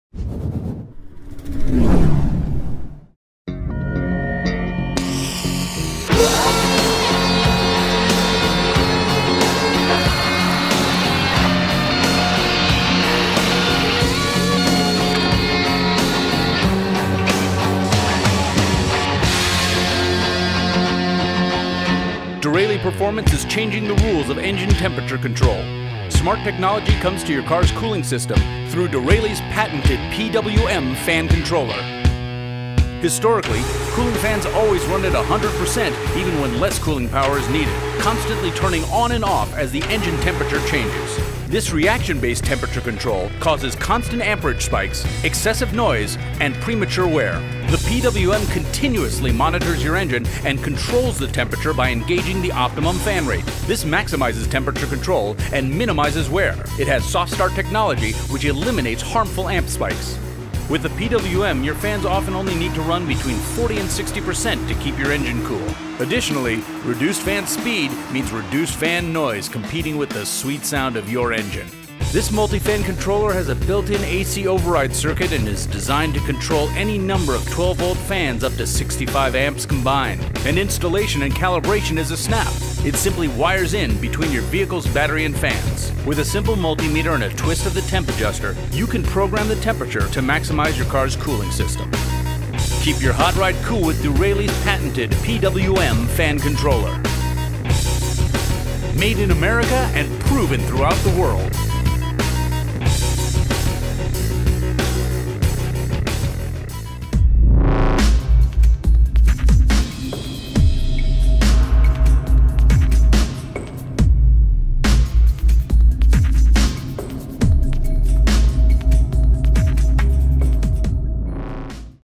Derale Performance PWM Electric Fan Controller
Derale Performance is changing the rules of engine temperature control with Smart Technology brought to your car's cooling system through Derale's Patented PWM Fan Controller.
The Derale PWM fan controller continuously monitors your engine's temperature and increases the vehicles cooling efficiency, while reducing the amps used to do so.